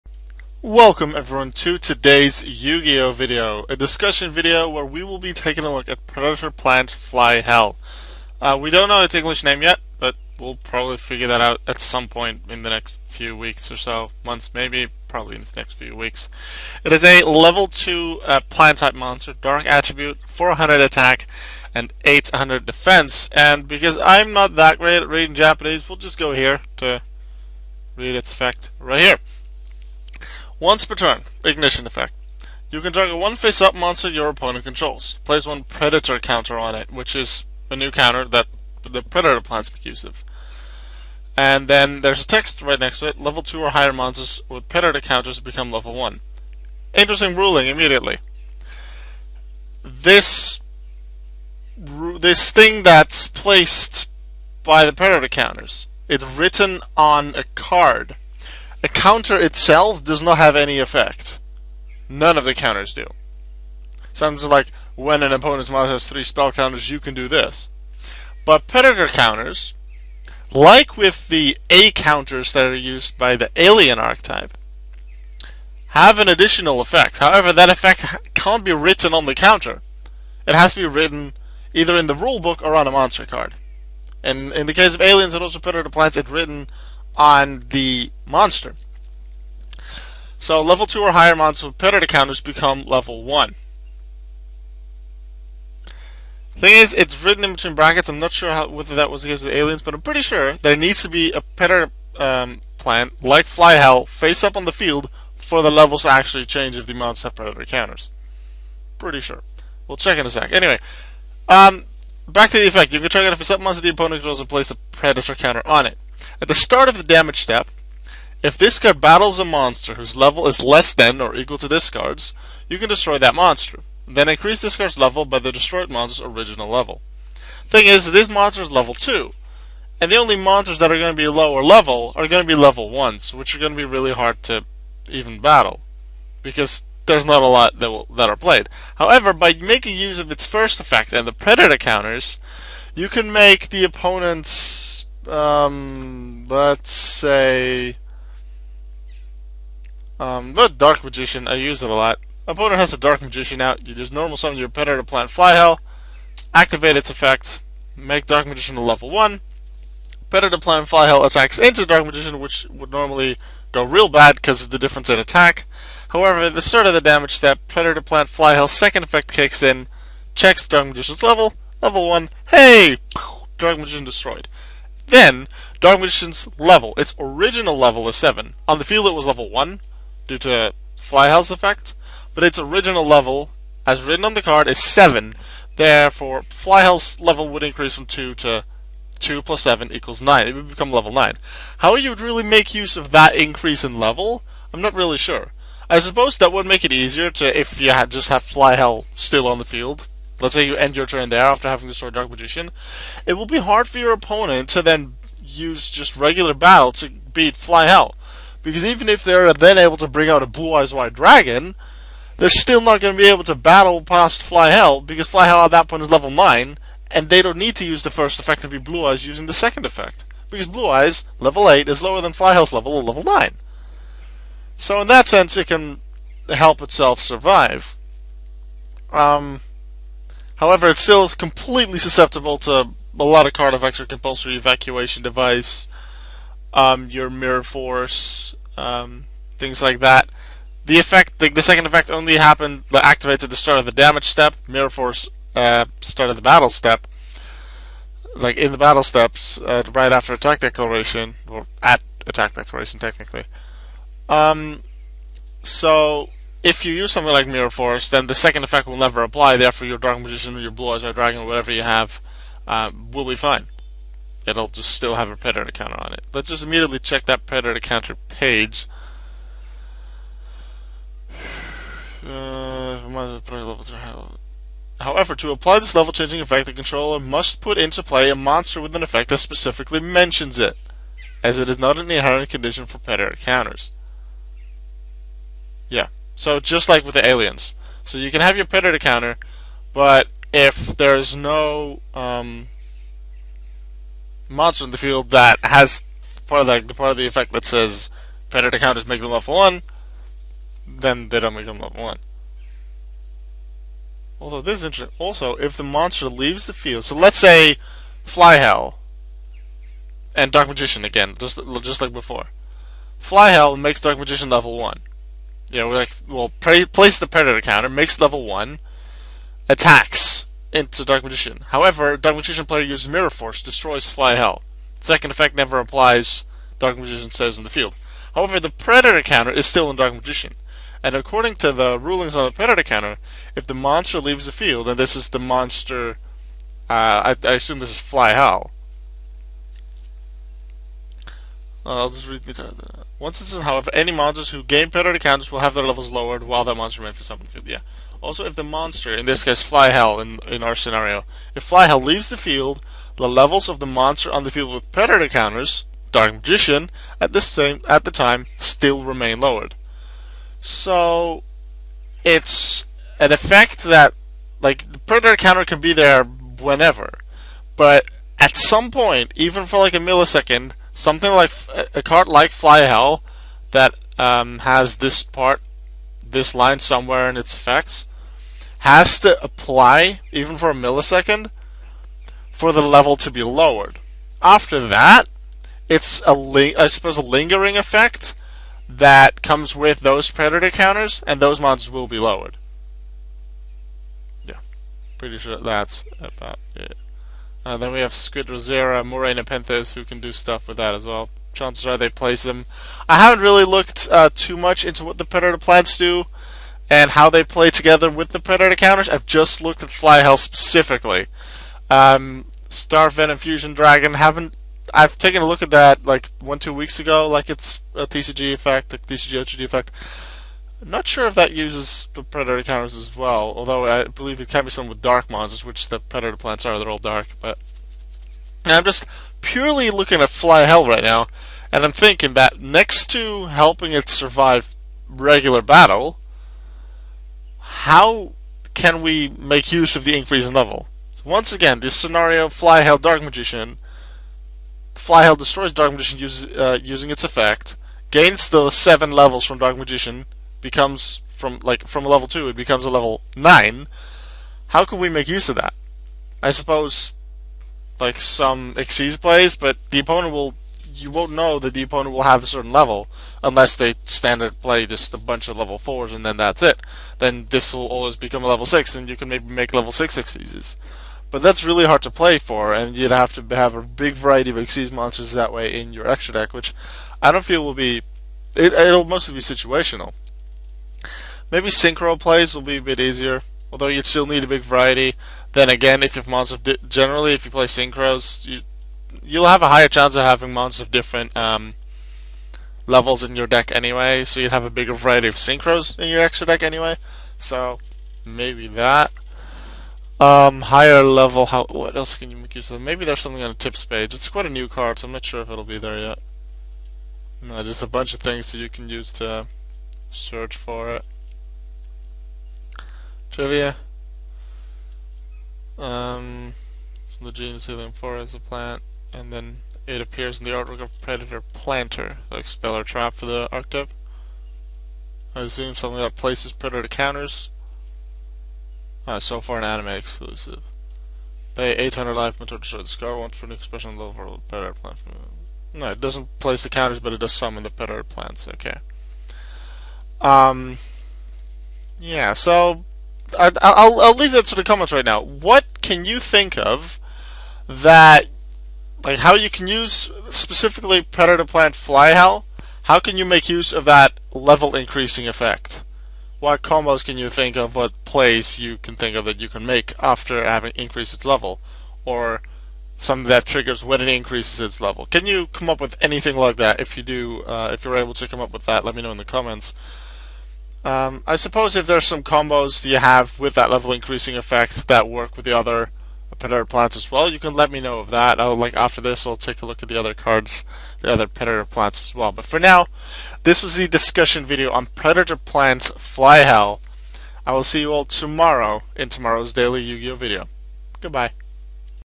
Yu-Gi-Oh Discussion #88: Predator Plant Fly Hell
We currently know of three members of the Predator Plant arcetype. In this video we're taking a closer look at one of them; Predator Plant Fly Hell.